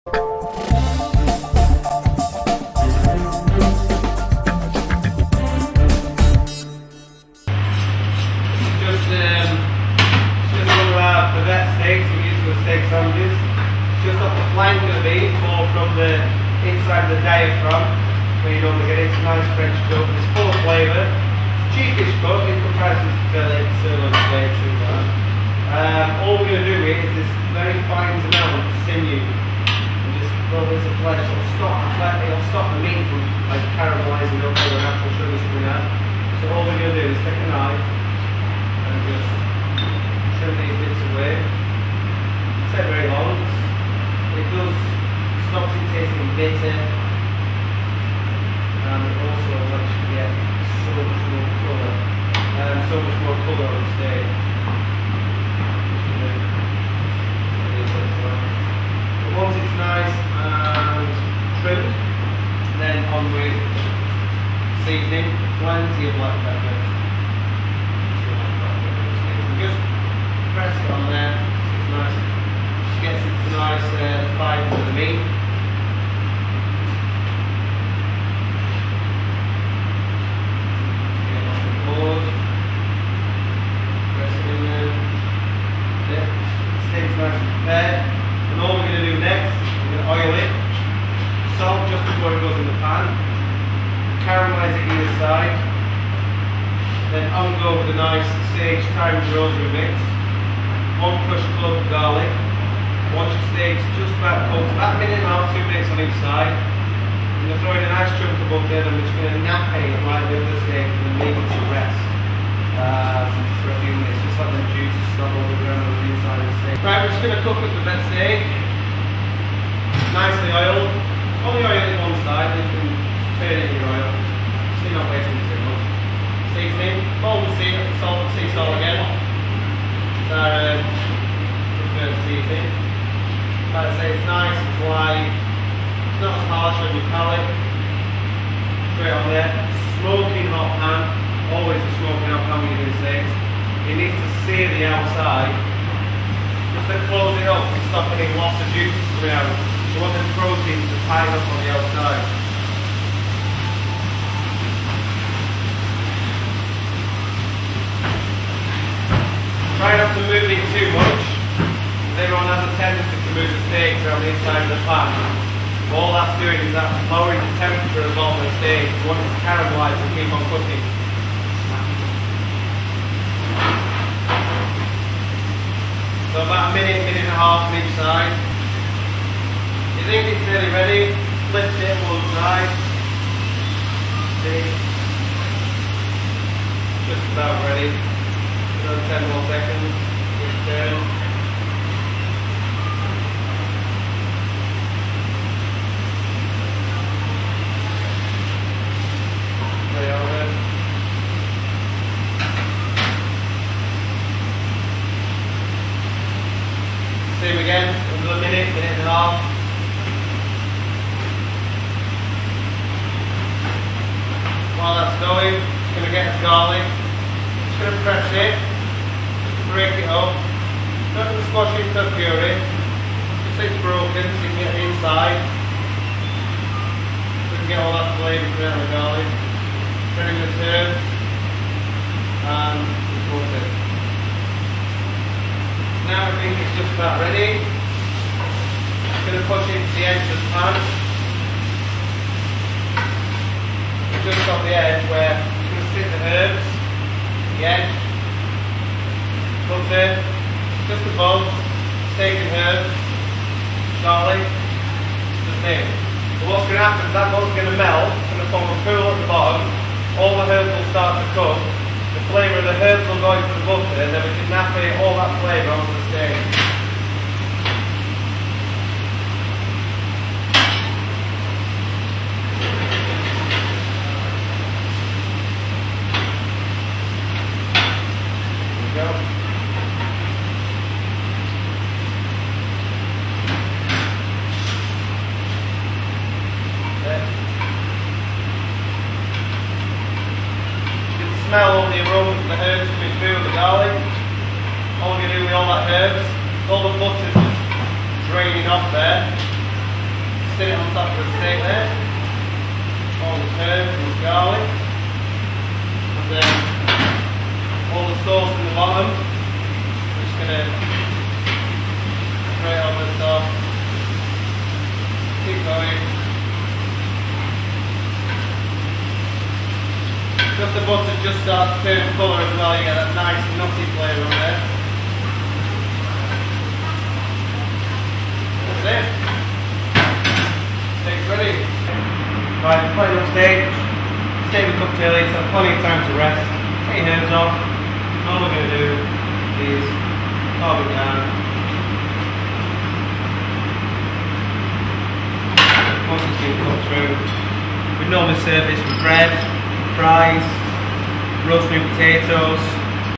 Preparing and Cooking a Steak with Simon Ayres
In this video you will see how a steak at the Showroom Cafe bar is prepared and cooked.

You will need:

Bavette Steak - cheap cut
Garlic
Butter
Salt+Pepper
Rosemary, Thyme, Sage

To start, trim the meat
Removing the sinew helps to improve colour and reduce bitter taste

Season to taste with plenty of Salt & Pepper

Sage, Thyme, Rosemary are perfect Herbs with steak!

Add oil directly to the meat

Ensure you have a smoking hot Pan! This will lock in all the flavour

Resist the urge to play with the meat as moving it around the pan will stop it cooking evenly.

Couple of Minutes each side

Press the Garlic, there's no need to smash

Add the garlic butter and herbs.

When melted all the flavour will be absorbed by the butter which we then coat over the meat.

As the butter starts to colour remove the pan from the heat and continue to coat the meat.

And there we have a lovely Bavette steak!